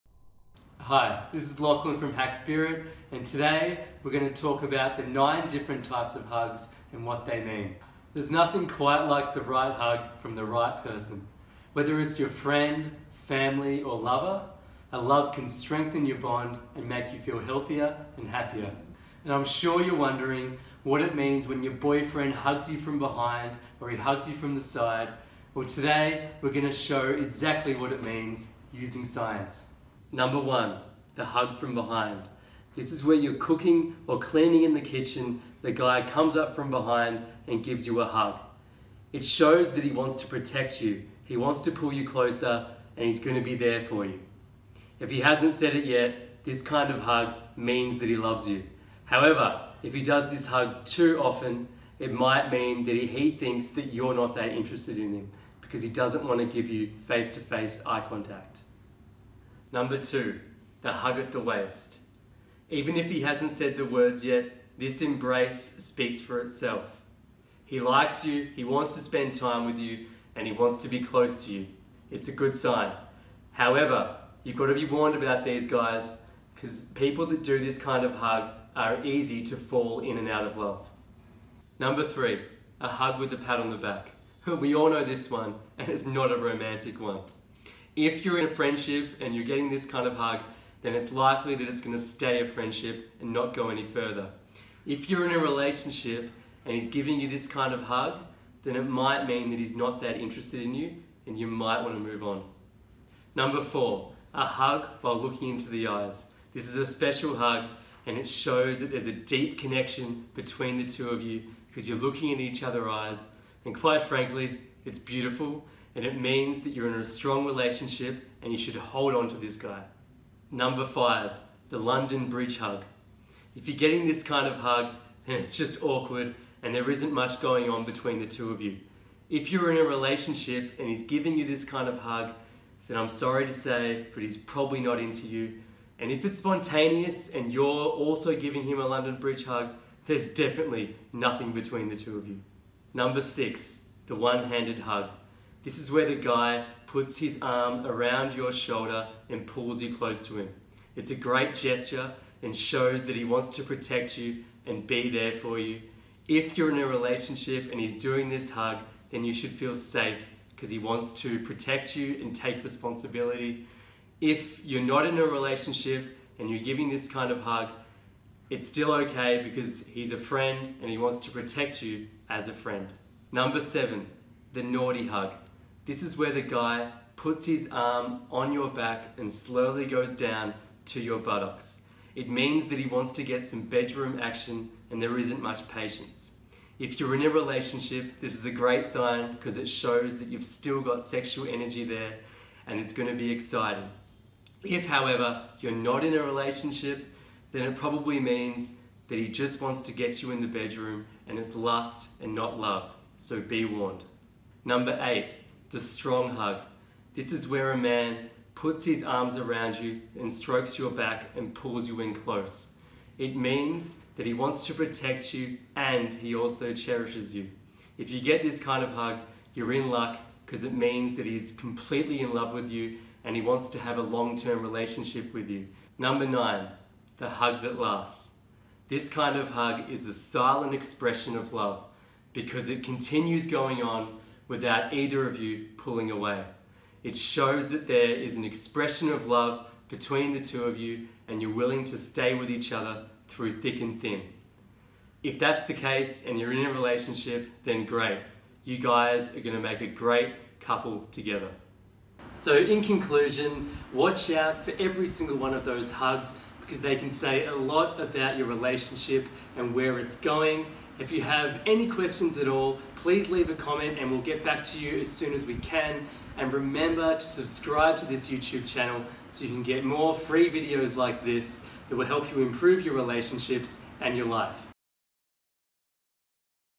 These 9 different types of hugs reveal what your relationship is really like
You may not have thought about it, but there are different types of hugs that convey different messages between people.

Hugs can say a lot about your relationship.

Here are what nine different types of hugs reveal.